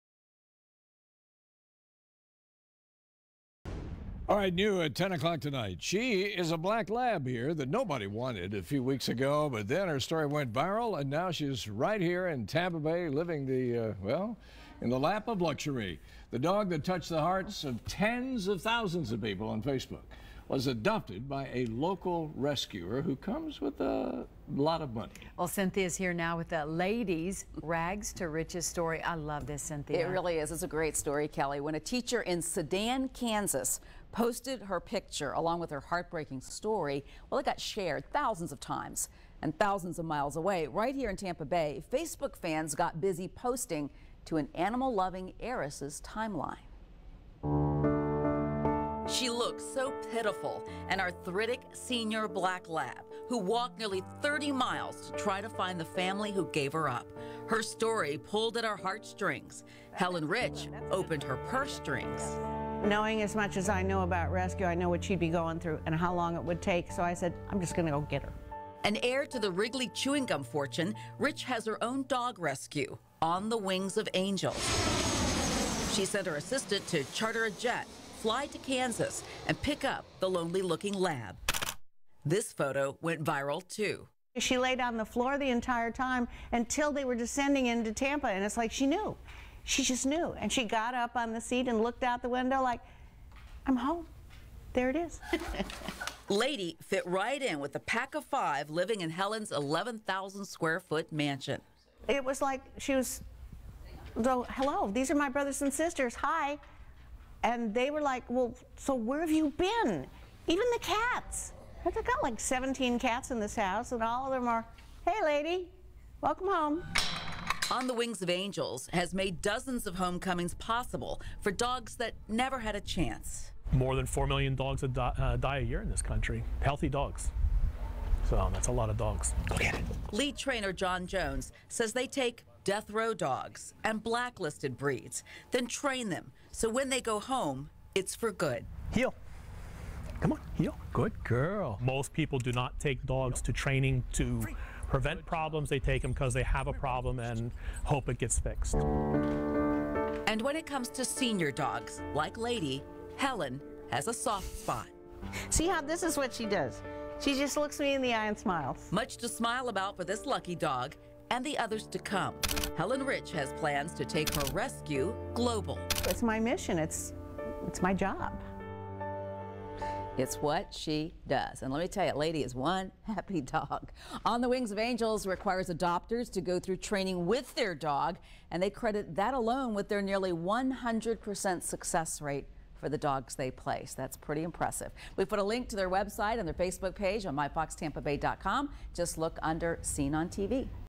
Abandoned dog, seen in viral photo, now living in luxury
She's the black lab nobody wanted a few weeks ago. But then her story went viral, and now she's right here in the Bay Area, living in the lap of luxury.